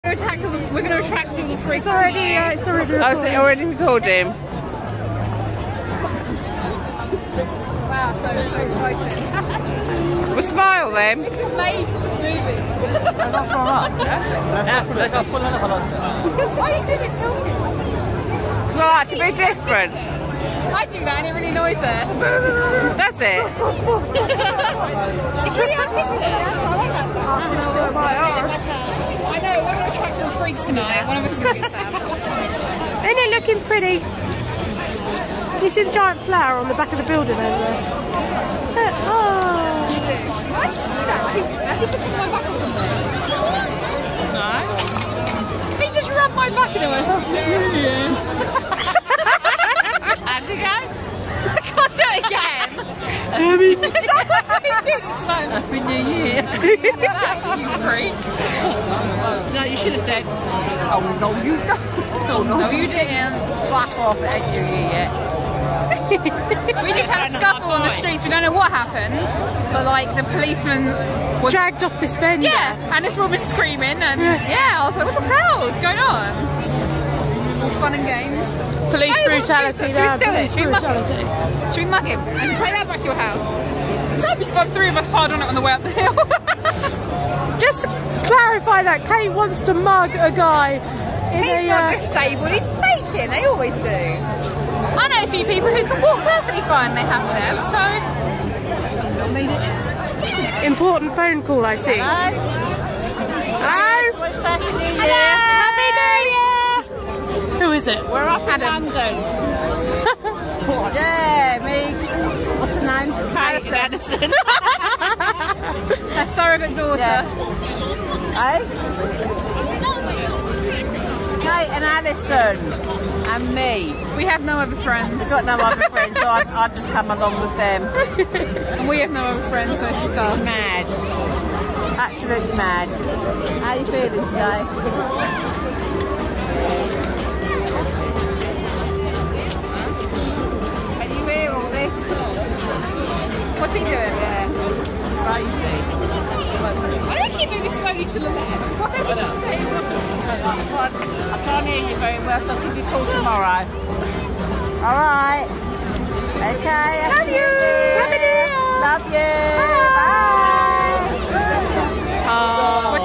Happy New Year 2009 from London Eye
Katie and me dancing and the first of many creepy blokes groping Katie before she suggests mugging a guy in a buggy, then a very important phone call.